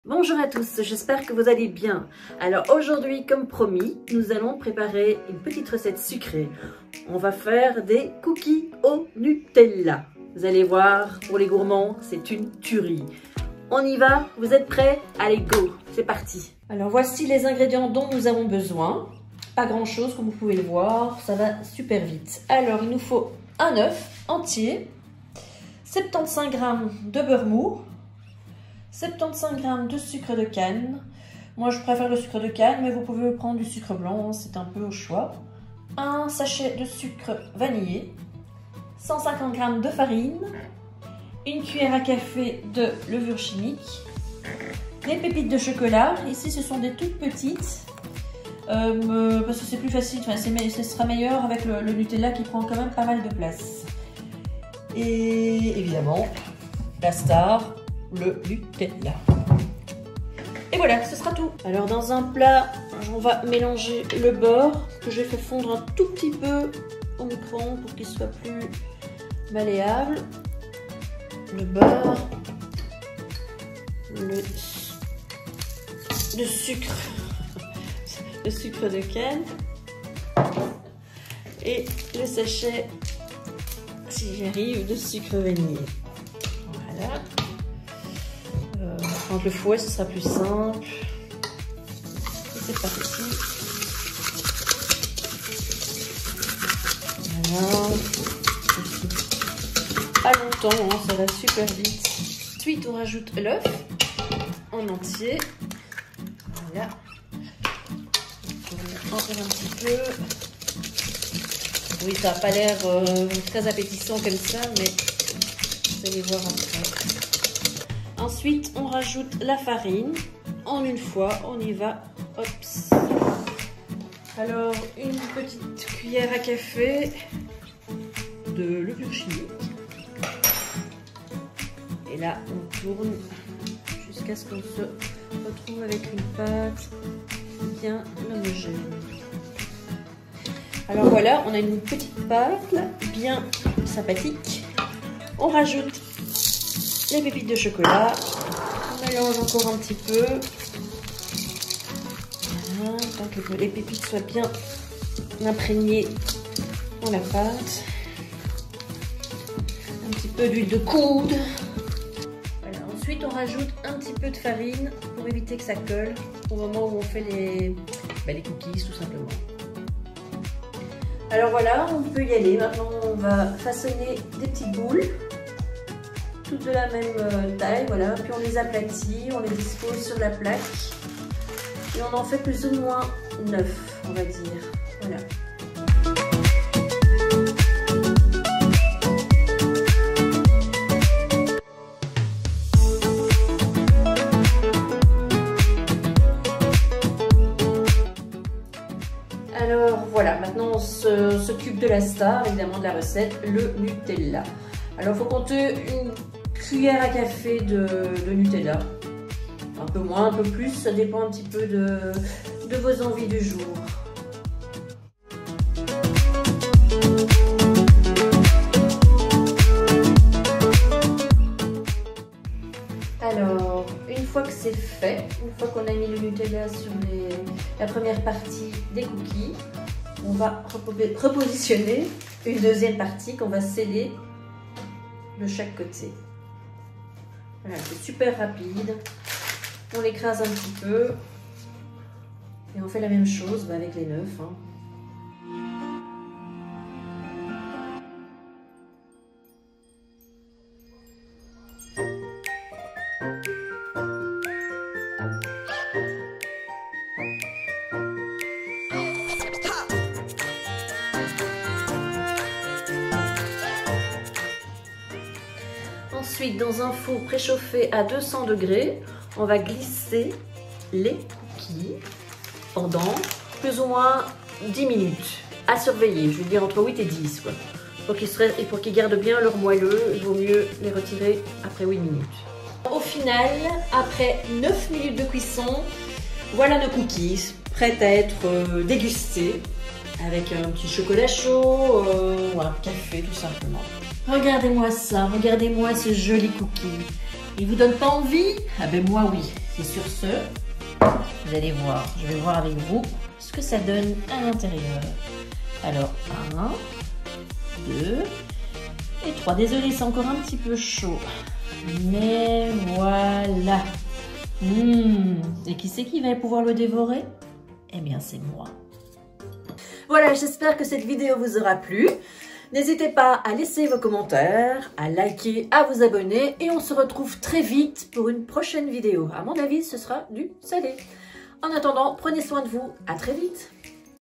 COOKIES au NUTELLA 🍪
Bonjour les amis ! Aujourd’hui cette recette va en plaire à plus d’un puisqu’elle est vraiment délicieuse : des cookies au nutella ! 🍪🍪

Une vraie vraie tuerie ! 😋

INGRÉDIENTS

-75g de beurre mou (un peu fondu)
-75g de sucre de canne (ou normal)
-1 sachet de sucre vanillé
-1 oeuf
-150g de farine
-1 cuillère à café de levure chimique
-100g de pépites
-nutella (+/- 1 cuillère à café par cookies)

Voilà c’est tout pour aujourd’hui, j’espère que cette recette vous plaira ! À très vite, prenez soin de vous les amis. 😁